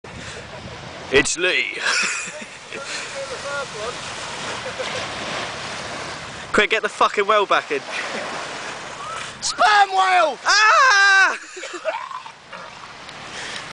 Sperm Whale
First time down the beach this year and a sperm whale came to shore!!